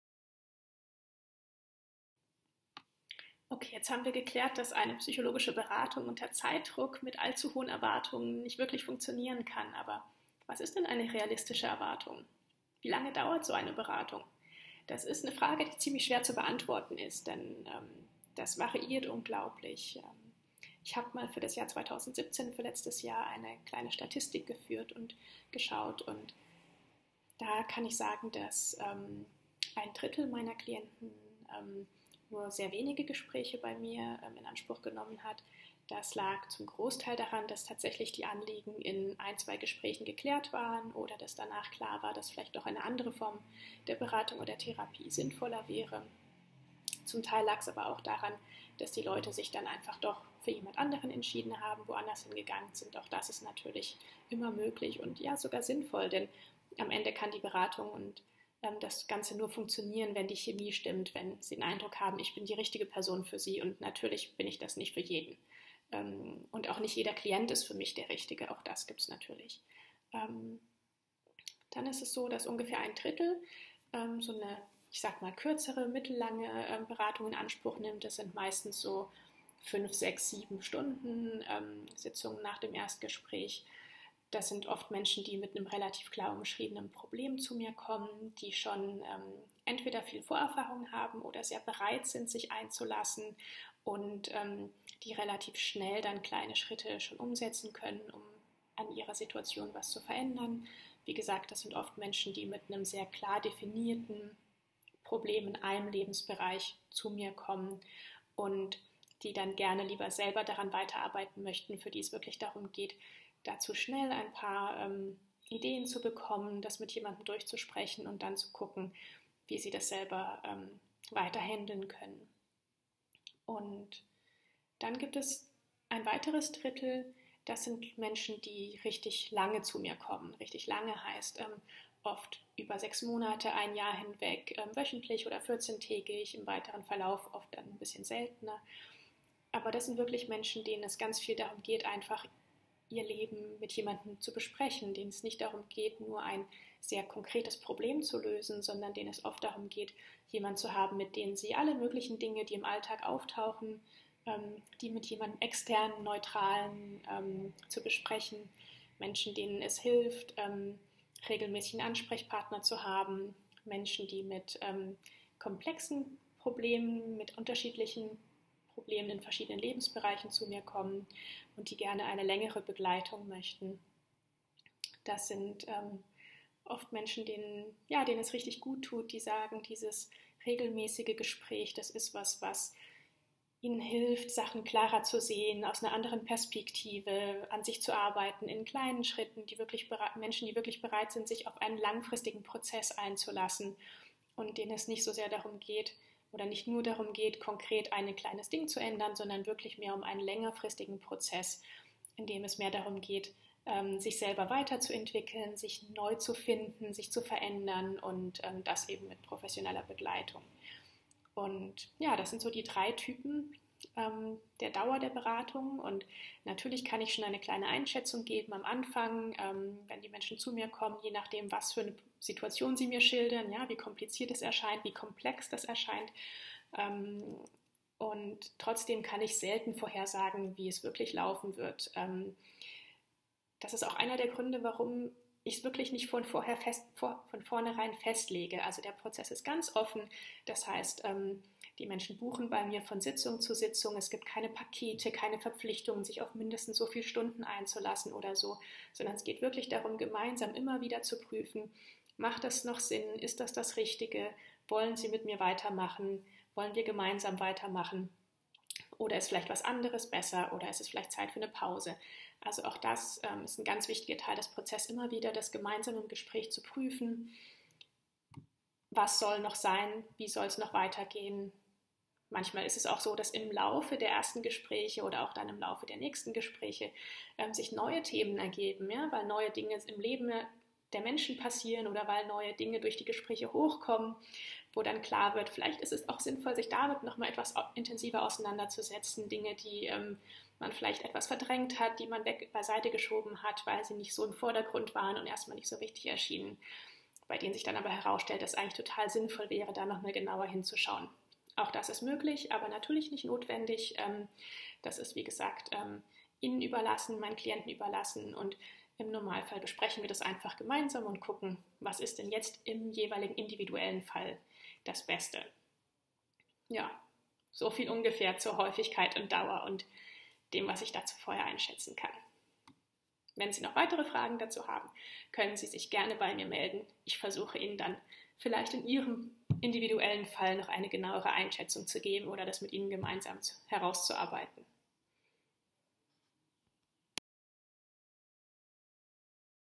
Wie lange dauert so eine psychologische Beratung eigentlich?
Eine Frage, die mir immer wieder gestellt wird, auf die es aber keine so ganz einfache Antwort gibt. Ich versuche es trotzdem und plaudere ein bisschen aus dem Nähkästchen einer Online Psychologin.